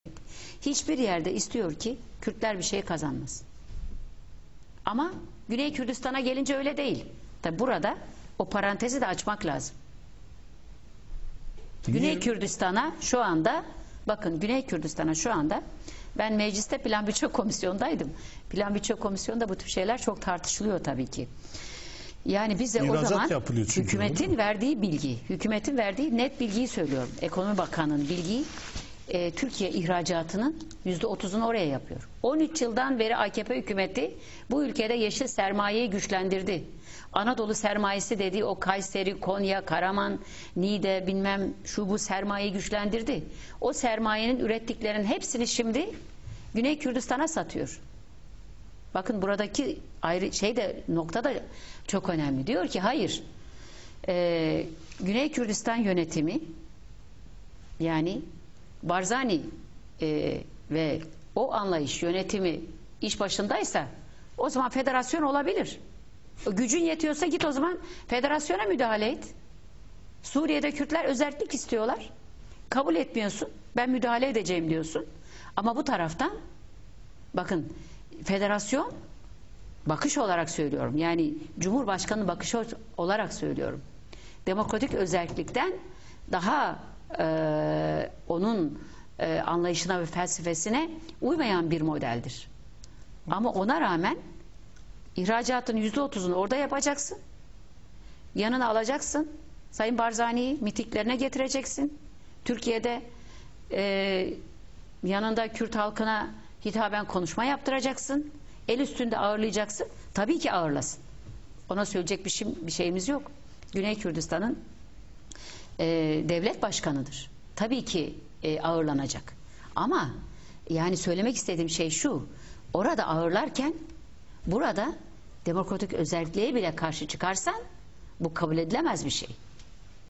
Nursel Aydoğan'ın çarpıtılan konuşması
Amed Milletvekili Nursel Aydoğan'ın çeşitli çevreler tarafından çarpıtılan konuşması. Siyasi rekabeti mertçe yapanlara özel video. Ahlaksız yalancılar zaten bakmaz bile.